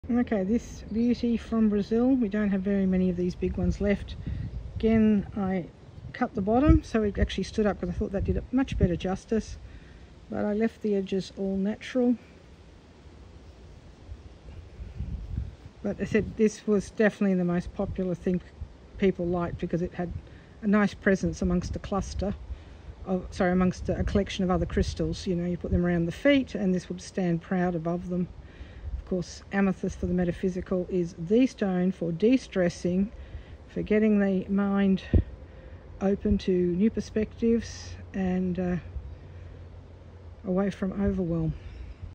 Need a gift that sparkles then look at this nice upright amethyst - SH106 amethyst
Brazilian amethyst cluster with cut base done by me as they may lovely statement pieces.
Learn about care of amethyst on our other channel: @Crystal-Experiences